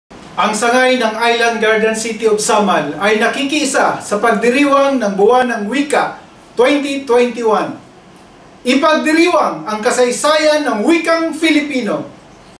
Mensahe ni OIC - Schools Division Superintendent Winnie E. Batoon, CESO VI | Division of IGACOS
Mensahe ni OIC - Schools Division Superintendent Winnie E. Batoon, CESO VI ng Sangay ng Island Garden City of Samal sa Buwan ng Wikang Pambansa 2021.